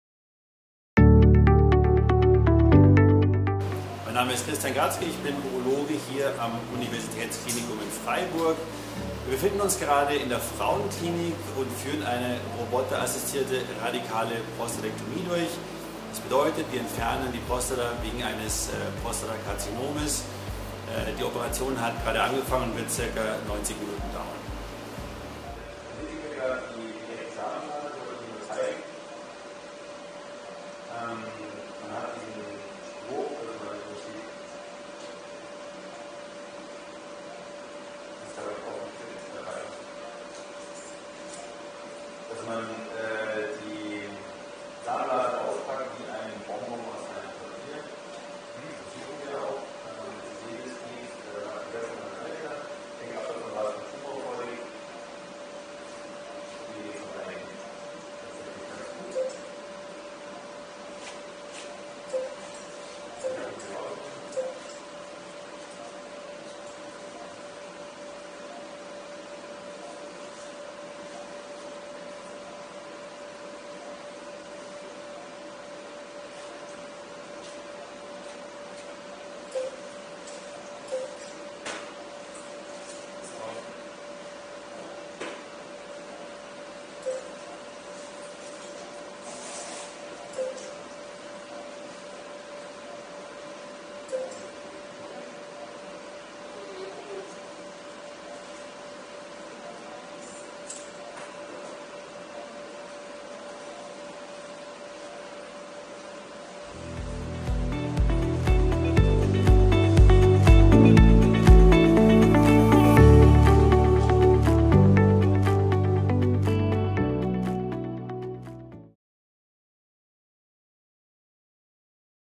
360-Grad DaVinci OP-Roboter | Uniklinik Freiburg
Spannende Einblicke in Bereiche des Klinikalltags, die sonst nicht frei zugänglich sind: Das bietet Ihnen die 360-Grad-Video-Reihe der Uniklinik Freiburg.

In der Klinik für Urologie am Universitätsklinikum Freiburg ist der modernste Da-Vinci-Operationsroboter seiner Art im Einsatz. Er ermöglicht unter anderem besonders schonende Prostata-Operationen.

Weitere Möglichkeiten der Uniklinik Freiburg zu folgen